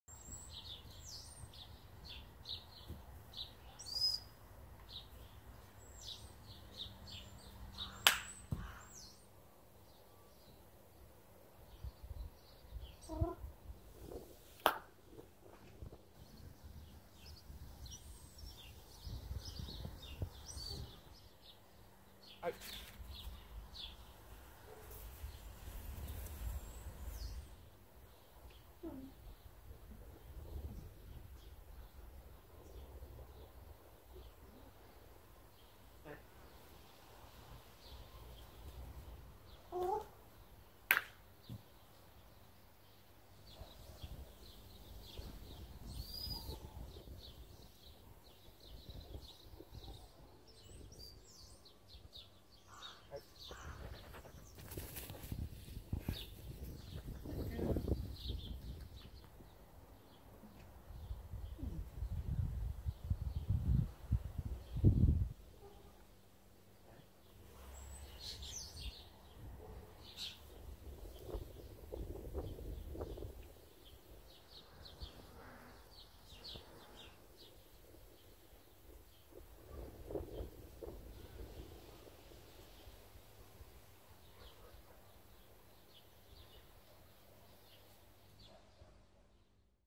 Dog Training | Left & Right Retrieves | Ruby
Showcasing Ruby doing left & right retrieves